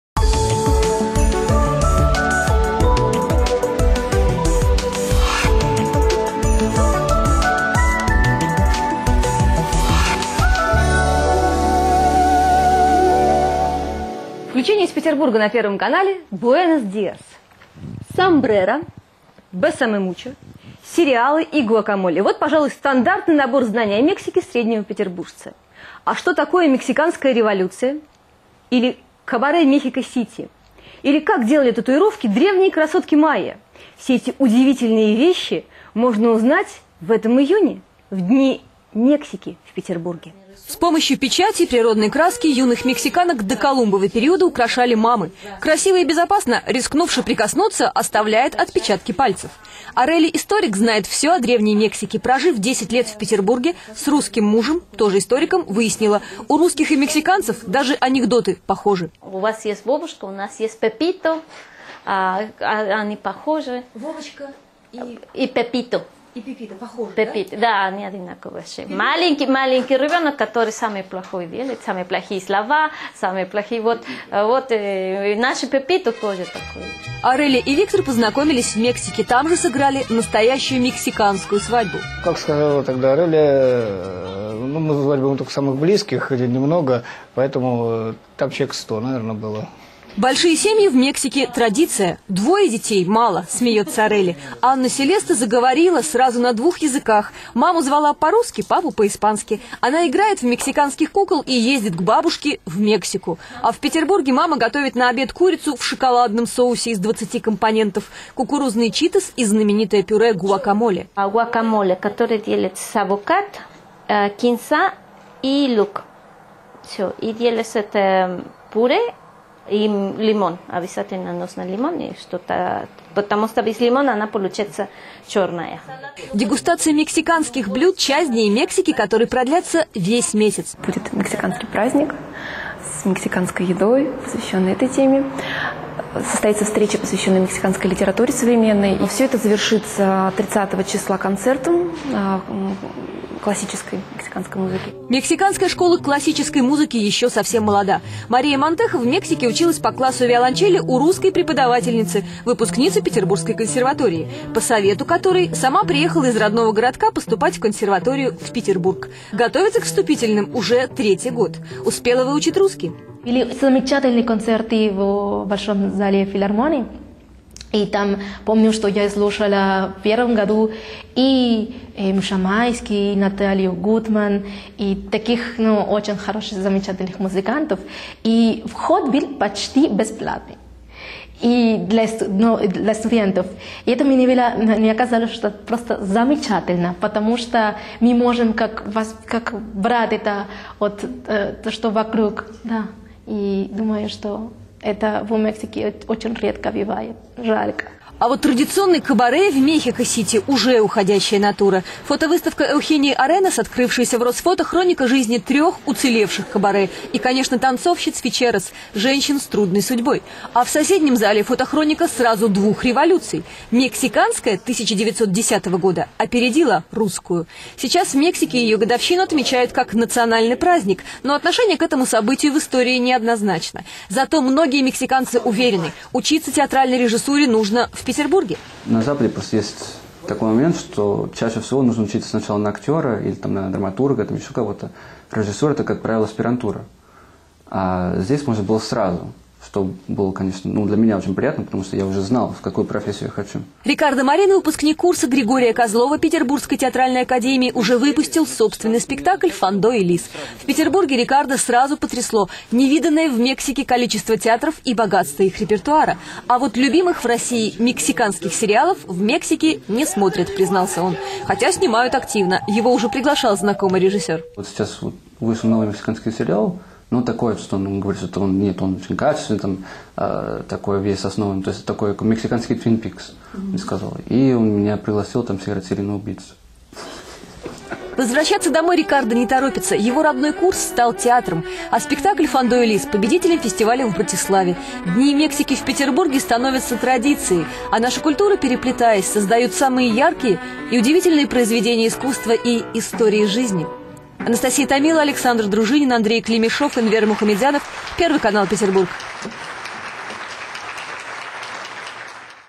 Mexicans in St. Petersburg/Mexicanos en San Petersburgo/Мексиканцы в Петербурге
Aired on June 9, 2011 on St. Petersburg's Channel 1 News, coinciding with the Days of Mexico in St. Petersburg 2011. In January 2010 another news piece ran on me, Ricardo Marin (director of "Fando and Lis")

Reportaje de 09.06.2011 pasado por el Primer Canal de San Petersburgo, coincidiendo con los Días de México en San Petersburgo 2011. En enero de 2010 otro reportaje sobre mí, Ricardo Marín (director de "Fando y Lis") fue pasado por otro canal

Репортаж Первого канала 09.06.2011, сделан в честь Дней Мексики в Петербурге. В январе 2010 г. вышел на эфир другой репортаж про меня, Рикардо Марин (режиссер "Фандо и Лис")

Translation beginning at 3:40:

In the neighboring space is a photochronicle of two revolutions at once: the 1910 Mexican Revolution came before the Russian one. Its anniversary in Mexico is celebrated as a national holiday, but people's relationship to that event in history is complex. Many Mexicans are convinced that one should study theatre directing in Petersburg.

"In the west it's necessary, more often than not, for one to study acting or playwrighting first." Ricardo Marin - Theater Director
"Directing is, as a rule, a master's degree. But here I could study it right away, which was, of course, very pleasant for me since I already knew which profession I wanted to go into."

Ricardo Marin, a graduate of Grigory Kozlov's class at the St. Petersburg State Theatre Arts Academy, has alread staged his own production of "Fando and Lis". In Petersburg Ricardo was shocked by the sheer amount of theater collectives and richness of their repertoire compared to Mexico. But he confesses that he didn't watch Mexican TV shows, which are so popular in Russia and which are still made regularly. He was already invited to be in one by a director he knows.

"He told me about this new show, which he said was different from the rest, that it was of much higher quality. Like a Mexican Twin Peaks, he said. And he invited me to play a serial kiler."

Ricardo is in no hurry to return home; his group from the Academy has become a theater, and his production of "Fando and Lis" was the winner at a festival in Bratislava.

These Days of Mexico in Petersburg are becoming a tradidition and our cultures, intertwining, are giving birth to the most bright and surprising works of art and life stories.

Anastasia Tamilo, Alexandr Druzhinin, Andrei Klimishev. Channel One Petersburg.